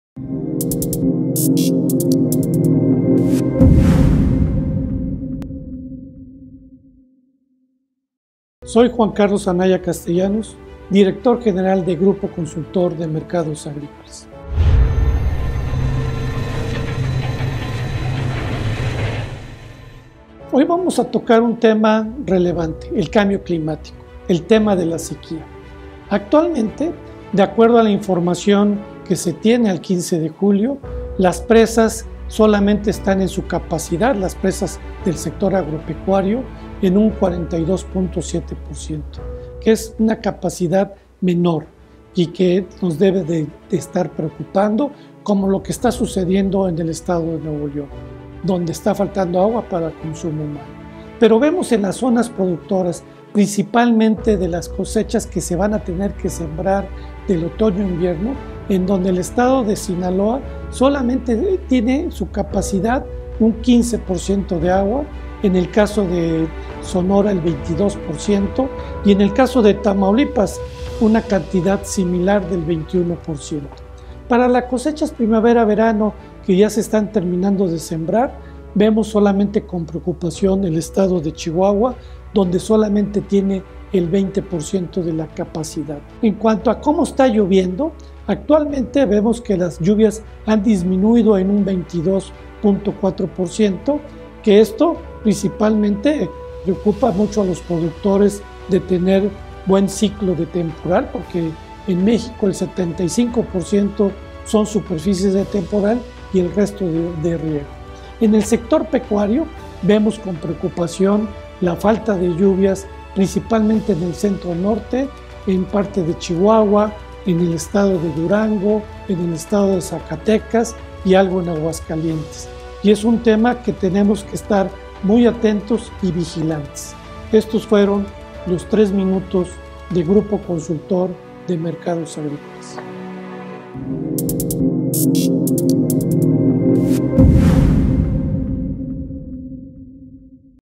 3 minutos con el GCMA: La sequía y el cambio climático, ¿cómo afecta al sector agroalimentario?
En 3 minutos con el Grupo Consultor de Mercados Agrícolas, Juan Carlos Anaya Castellanos explica los efectos que la escasez de agua y las variaciones en el clima tienen en la producción agroalimentaria.

La temporada de sequía ha afectado a la mayoría de las presas en México, de las cuales depende sector agroalimentario, principalmente en los estados con una importante producción agroalimentaria a nivel nacional, como Sinaloa, Sonora y Tamaulipas, que actualmente cuentan con menos de un cuarto de la capacidad hídrica en almacenamiento.

La falta de agua y el cambio climático, menor disponibilidad de pastos y forrajes apra el sector pecuario

La escasez del líquido obedece a la significativa reducción de las lluvias, cuya recurrencia ha bajado en este año por el orden del 22% en comparación con 2021.

Esto, a su vez, se ha traducido en desafíos para el segmento pecuario, debido a la menor disponibilidad de pastos y forrajes.

Derivado de las posibles disrupciones en los precios de los insumos y ante la falta de recursos hídricos, Juan Carlos Anaya, director general del Grupo Consultor de Mercados Agrícolas, comenta en  3 minutos con el Grupo Consultor de Mercados Agrícolas sobre este tema y sus implicaciones.